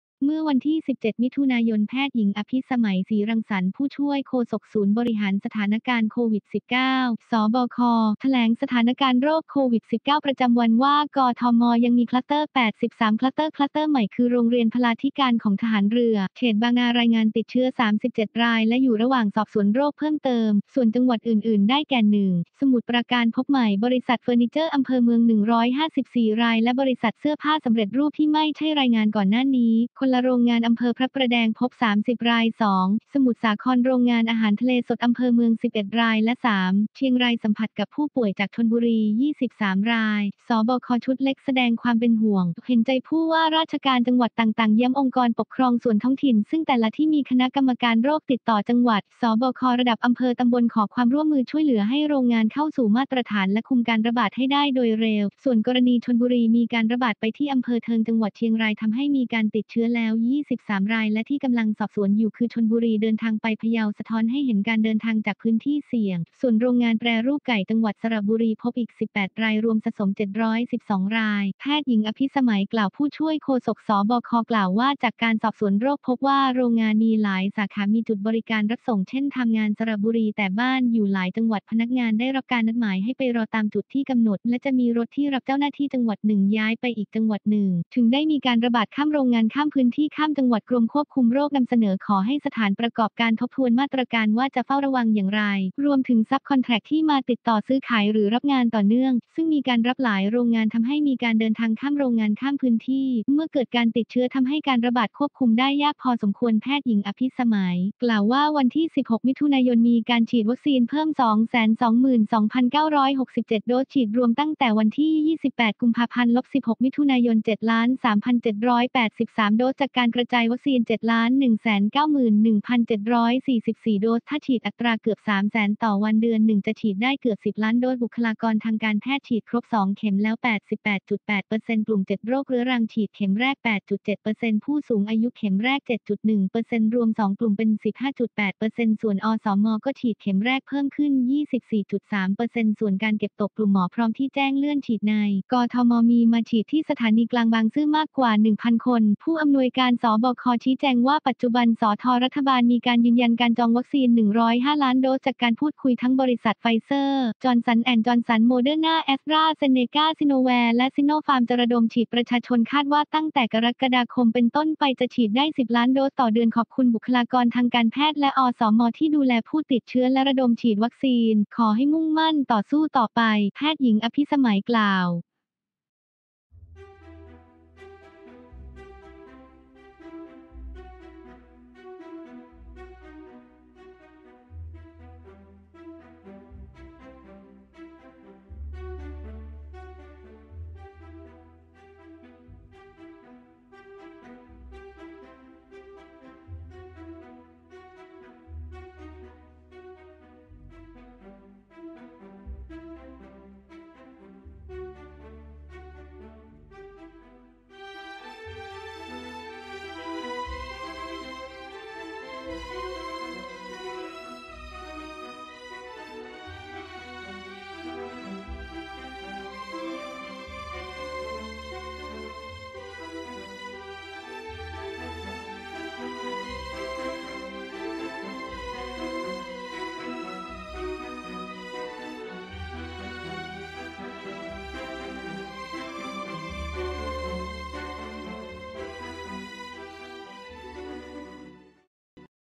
กทม.เจอคลัสเตอร์ใหม่ ร.ร.พลาธิการทหารเรือติด 37 ราย
เมื่อวันที่ 17 มิ.ย. พญ.อภิสมัย ศรีรังสรรค์ ผู้ช่วยโฆษกศูนย์บริหารสถานการณ์โควิด 19 (ศบค.) แถลงสถานการณ์โรคโควิด 19 ประจำวัน ว่า กทม.ยังมีคลัสเตอร์ 83 คลัสเตอร์ คลัสเตอร์ใหม่คือโรงเรียนพลาธิการของทหารเรือ เขตบางนา รายงานติดเชื้อ 37 ราย และอยู่ระหว่างสอบสวนโรคเพิ่มเติม ส่วนจังหวัดอื่นๆ ได้แก่ [...]